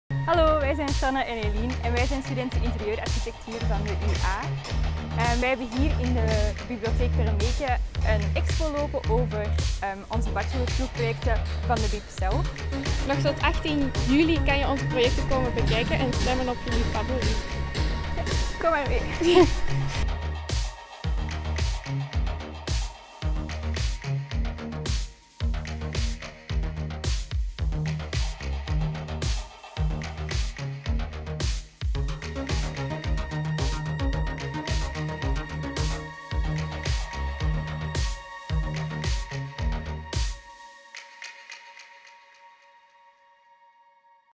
Sanne en Elien stellen hun bachelorproef voor in Bibliotheek Permeke
Benieuwd naar het project van Sanne, Elien en de anders studenten interieurarchitectuur (UAntwerpen)? Bezoek nog tot 18 juli Bibliotheek Permeke en breng je stem uit voor één van de projecten.